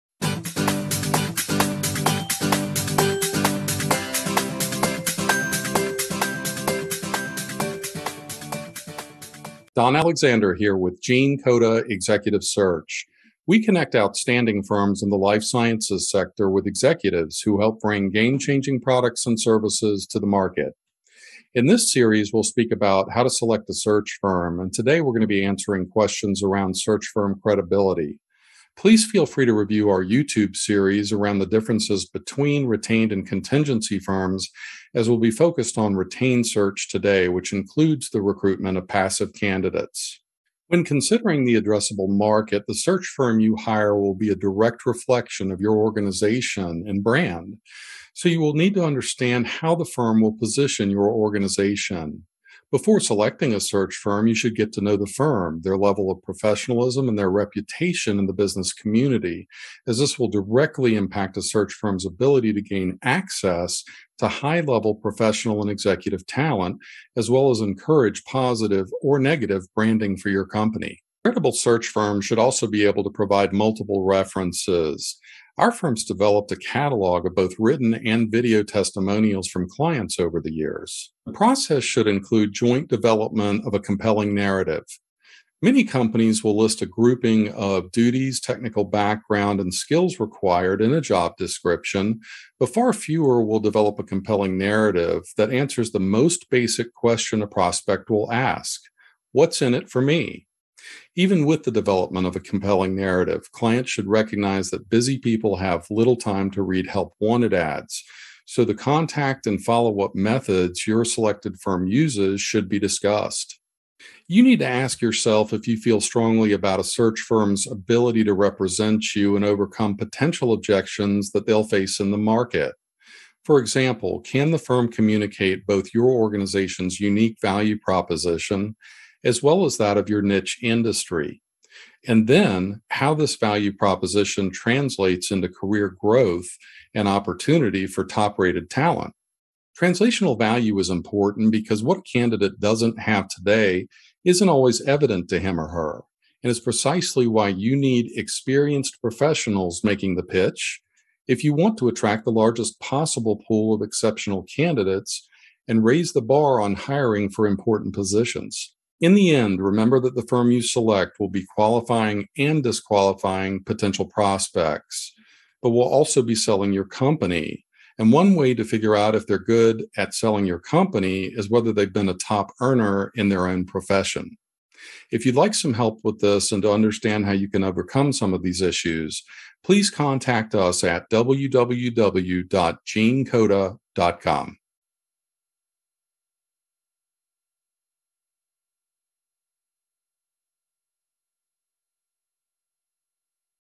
How to select a Search Firm - Part 2
In this series, we’ll speak about how to select a search firm and this episode covers questions around search firm credibility.